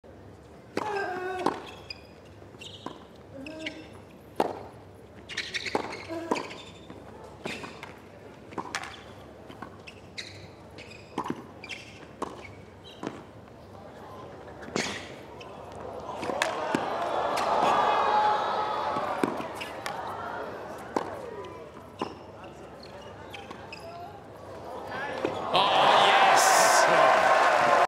INCREDIBLE point! 👏
Daniel Altmaier wins an incredible rally against Karen Khachanov in the first round of the Australian Open 2024. krakenexchange epicrallies @KrakenCryptoExchange